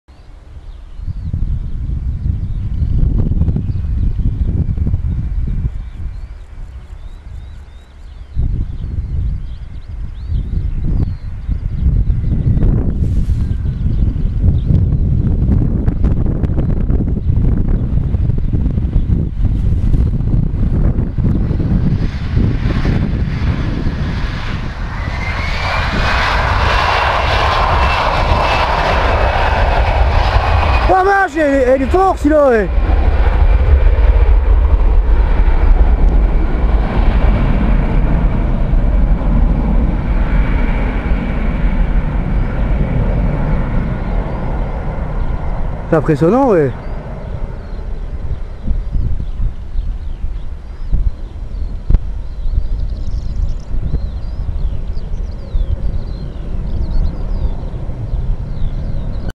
Swiss International Airlines A320-214 touch and go
L'Airbus A320-214 Swiss International Airlines à s'entraine à l'aéroport d'Epinal-Mirecourt